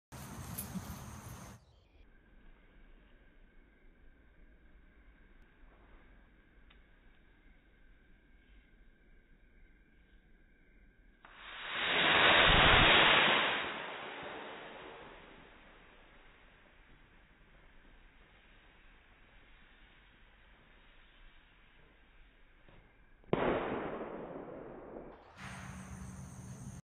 Bottle Rocket In Slow Motion?
Kevin and I having fun launching fireworks last Independence Day. I have a few more of these videos. Didn't make last week's upload (just started a new job). Sorry about that.